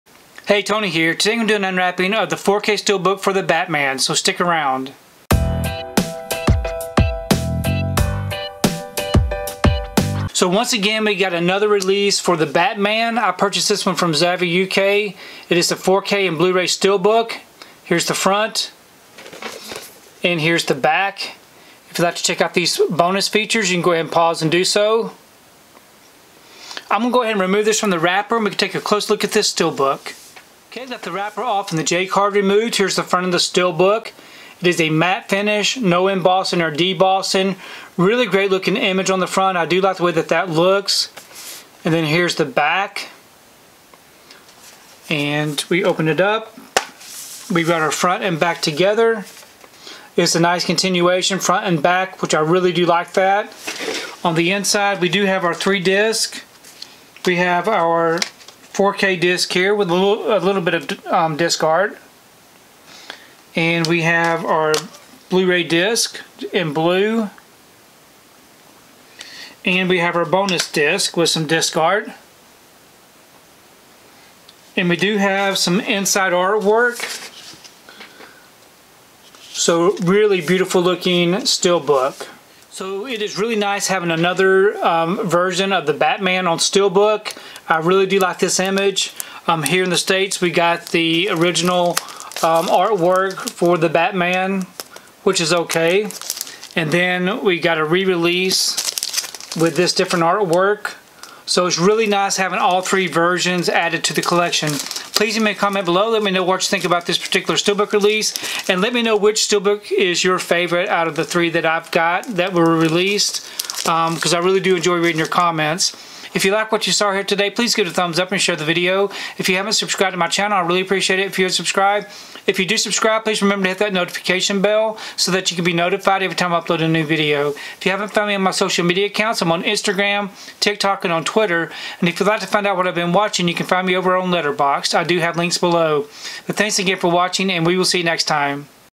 The Batman 4K Ultra HD Blu-ray Steelbook
Check out the 4K Ultra HD Blu-ray Steelbook for The Batman that I purchased from Zavvi UK!

► Add the 4K Ultra HD Blu-ray to your collection
► Add the Blu-ray to your collection